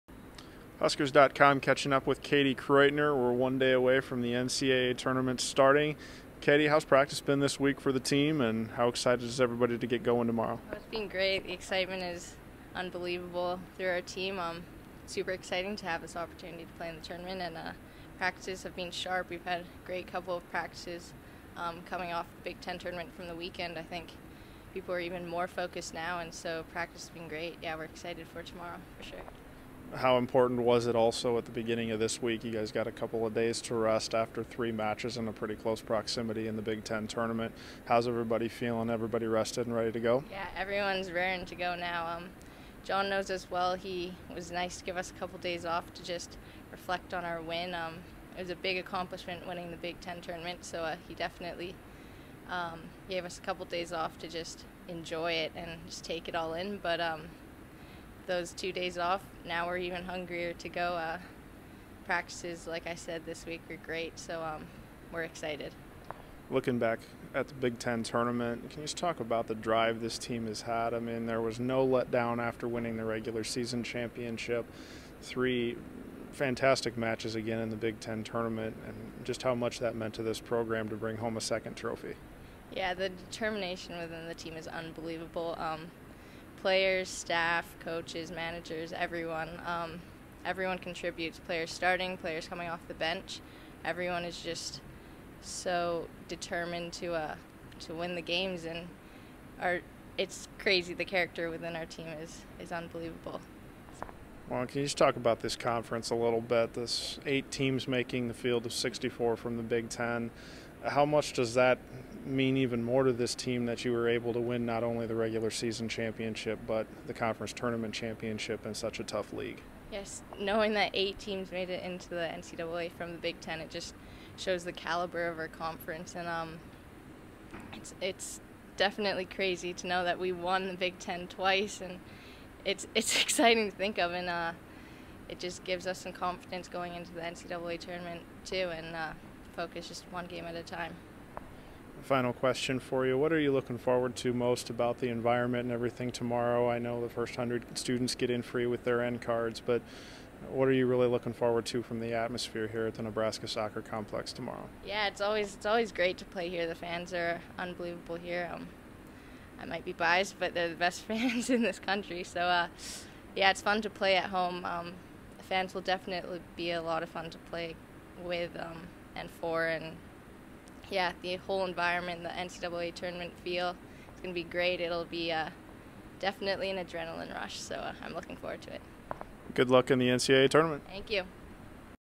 Katie Kraeutner Interview 11-14-13
Katie Kraeutner talks with huskers.com before the Huskers open NCAA Tournament play against Southeastern Louisiana.